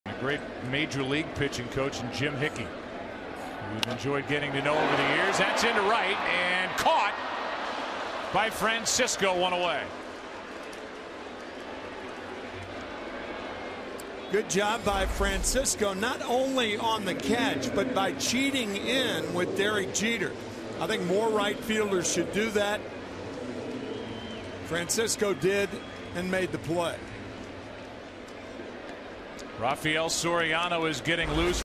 2012/09/15 Francisco's sliding catch
TB@NYY: Francisco slides for a great grab in right